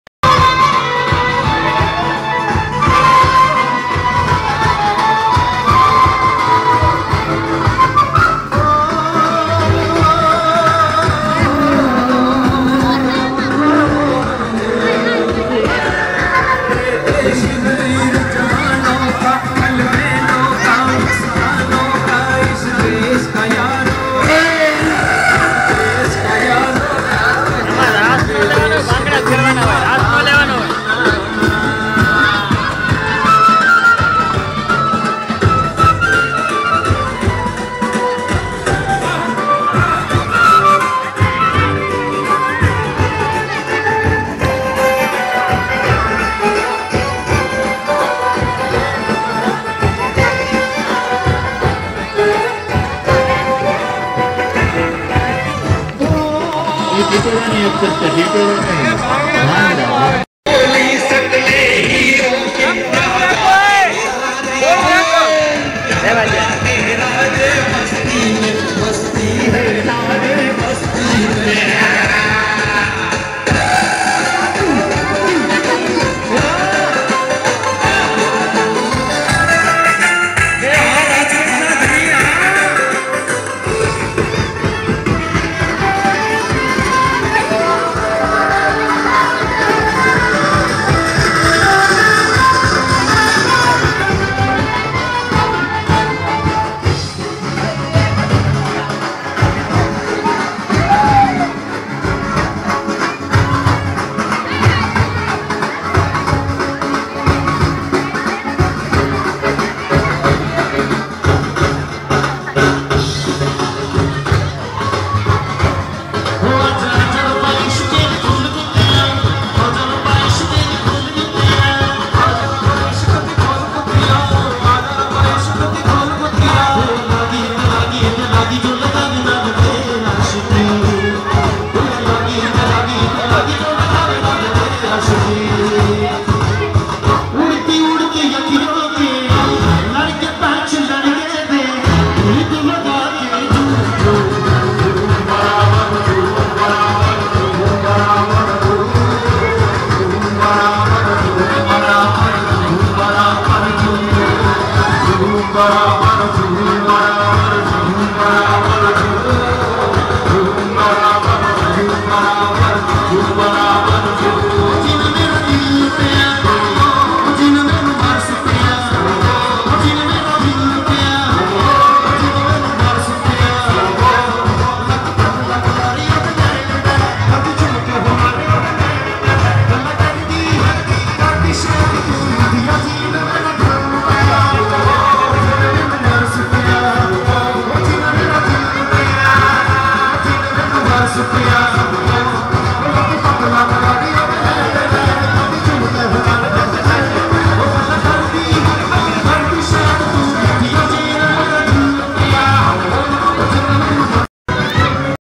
navratri mahotsav in school jira.52.168_ part 11
navratri mahotsav in saradar patel madhyamik school, jira. amreli.
navratri ras harifai in village jira,11